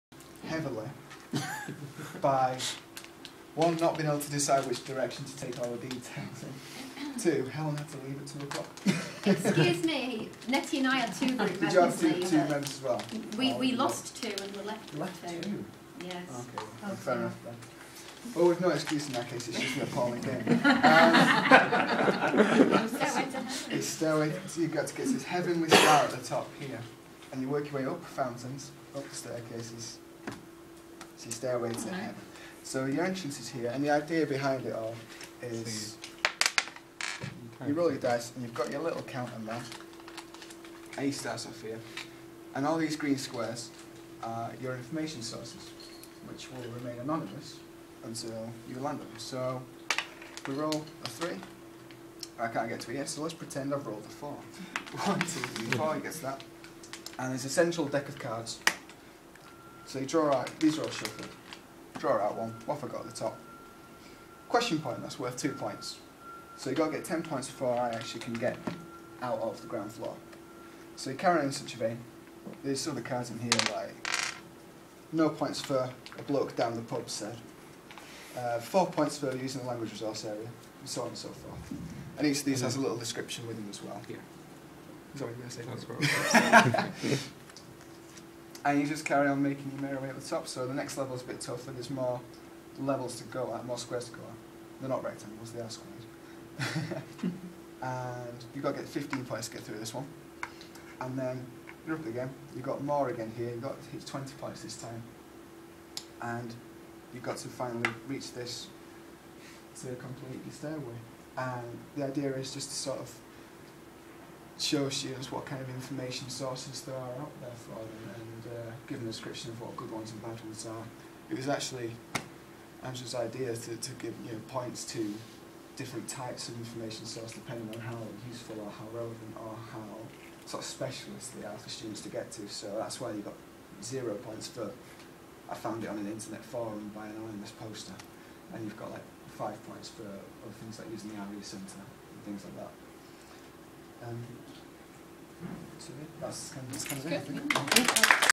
Stairway to heaven
A board game aiming to show the value and range of information sources available to students.  A prototype from Making Games for Libraries workshop run at York St John University, November 2013.